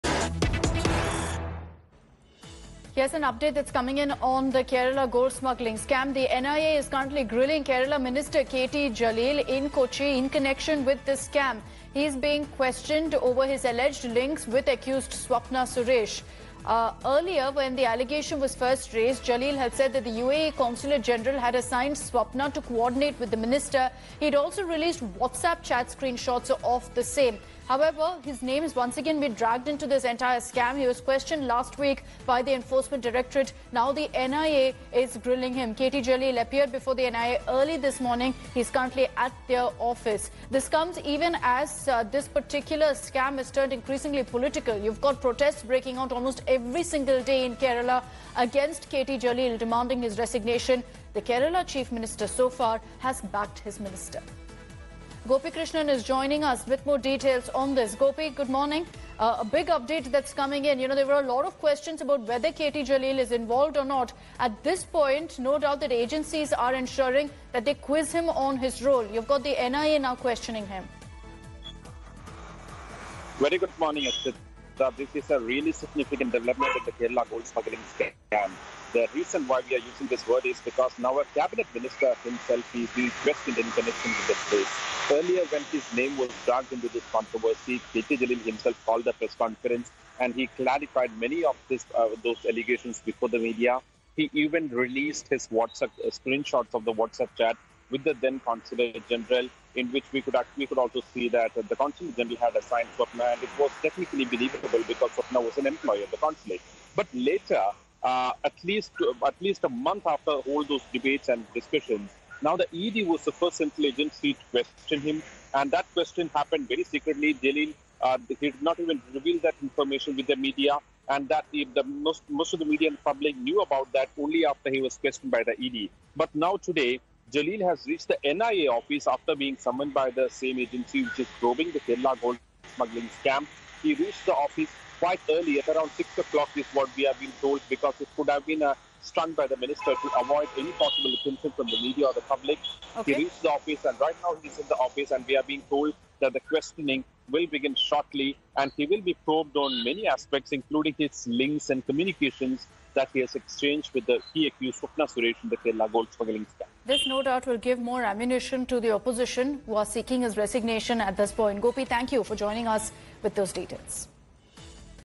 Kerala Gold Smuggling Case: NIA Probes Minister KT Jaleel Over Alleged Link To Accused Swapna Suresh
Kerala Higher Education Minister KT Jaleel reached the National Investigation Agency's office in Kochi today morning for questioning, a week after he was quizzed by the ED.

India Today LIVE TV | Watch Latest News In English | Breaking News Live

Watch the latest live English news and updates on India Today.